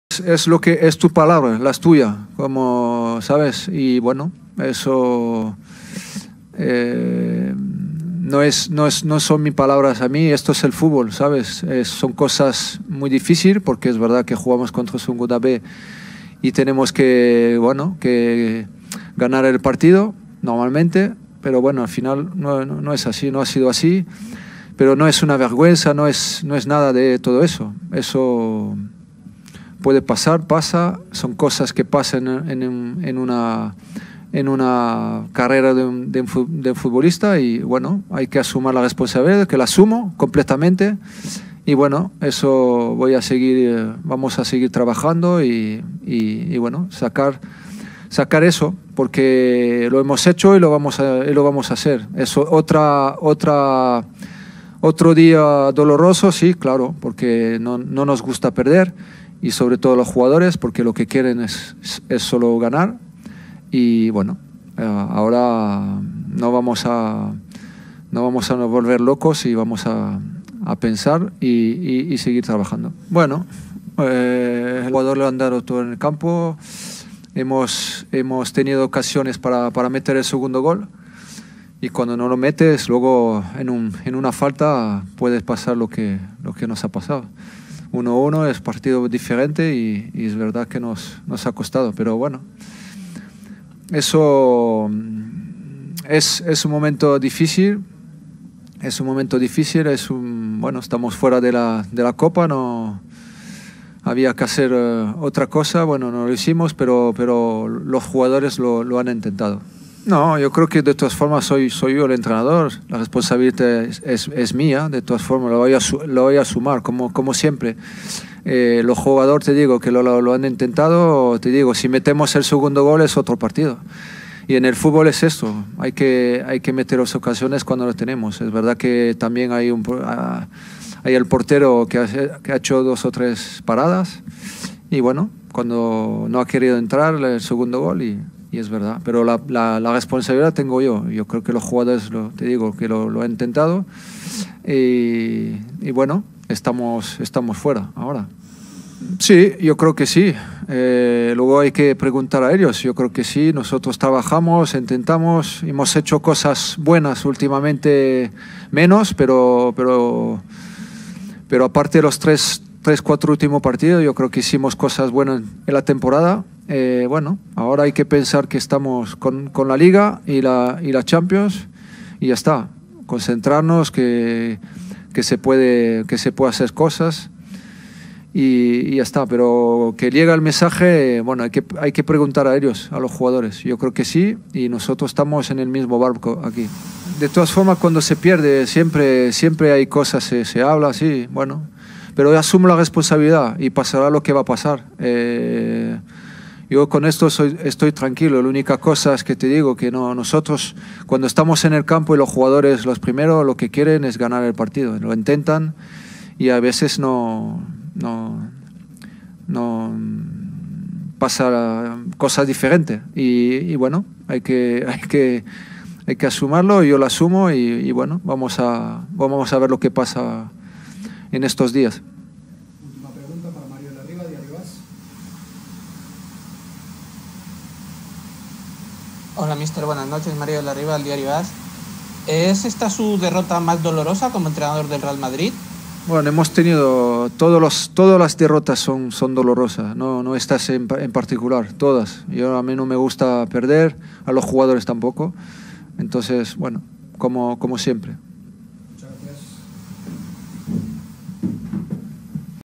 "No es una vergüenza, es otro día doloroso": Zinedine Zidane │Real Madrid │Copa de Rey │
En conferencia de prensa, el estratega francés del Madrid aseguró que la derrota contra el Alcoyano no es una vergüenza, ya que sus futbolistas lo dieron todo en el terreno de juego y ese esfuerzo se debe de agradecer.